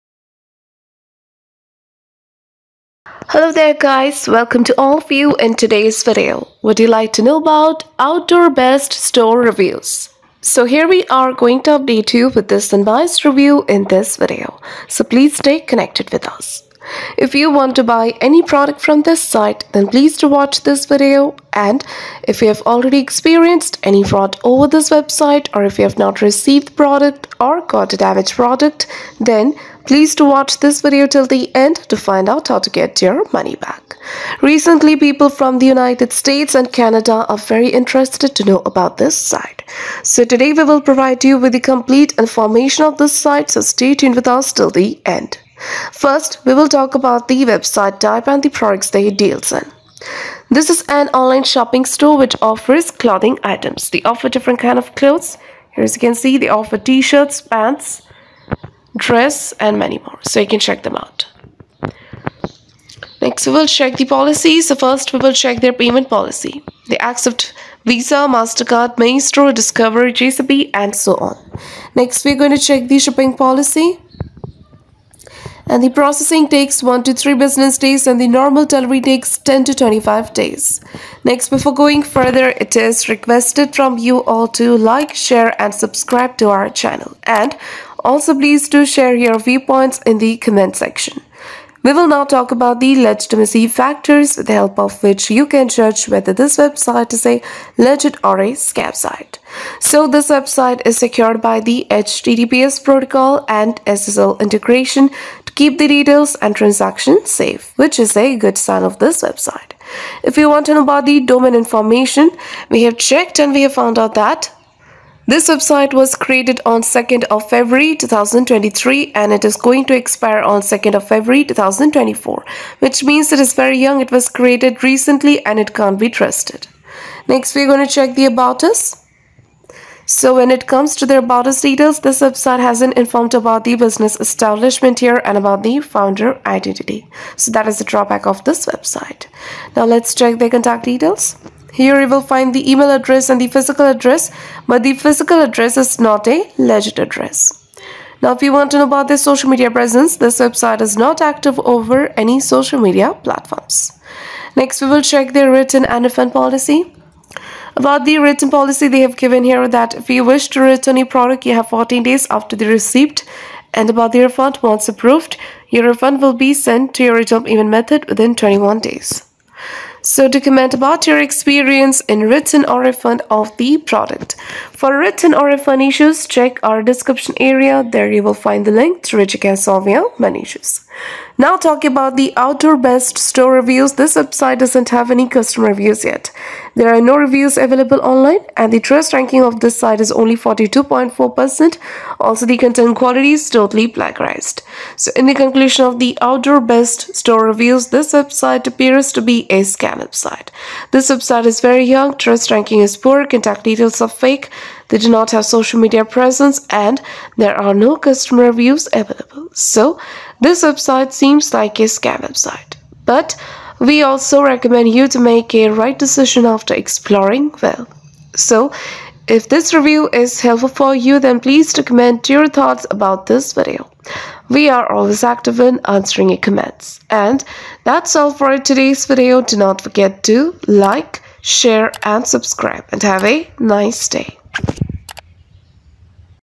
Outdoorbest Store Reviews (March 2023) - Is This A Legit Or A Scam Site? Find Out! | Scam Inspecter
Do you want to know about Outdoorbest Store Reviews?  Watch this video till end because in this video you will get to know about the legitimacy of the site.

Scam Inspecter